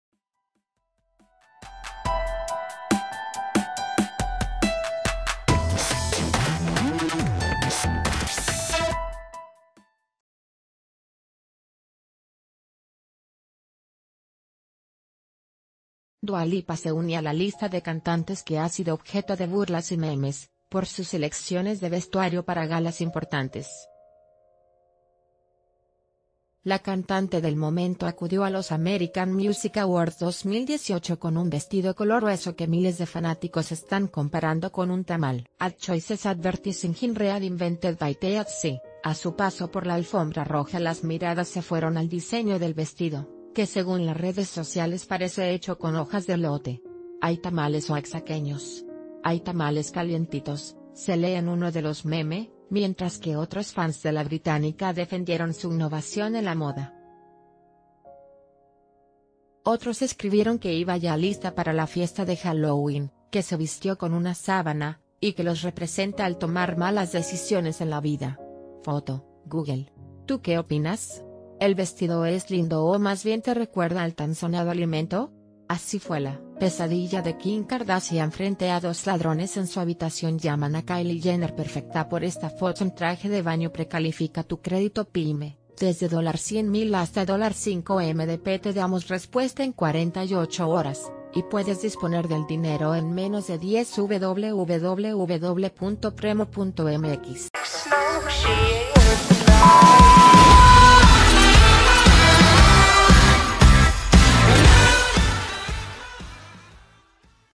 Comparan a Dua Lipa con un tamal oaxaqueño al aparecer en alfombra roja
Dua Lipa se une a la lista de cantantes que ha sido objeto de burlas y memes, por sus elecciones de vestuario para galas importantes. La cantante del momento acudió a los American Music Awards 2018 con un vestido color hueso que miles de fanáticos están comparando con un tamal. AdChoicesADVERTISINGi...